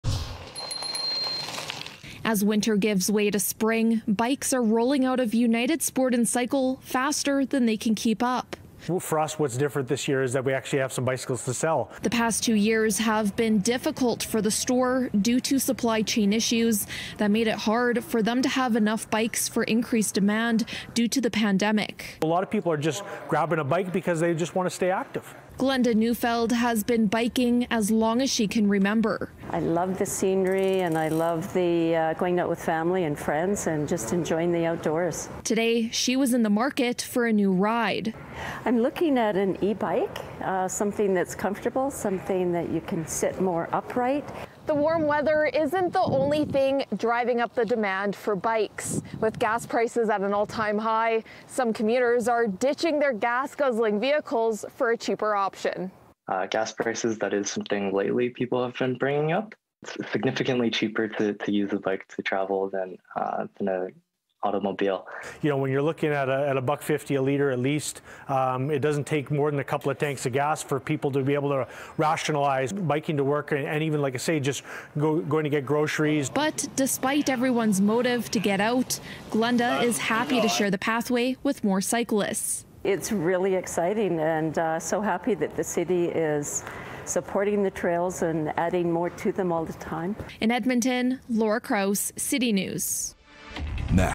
Demand for bikes continues to rise in Edmonton
As winter gives way to spring, bikes are rolling out of United Sport and Cycle faster than they can keep up. Laura Krause explains why bikes are in high demand.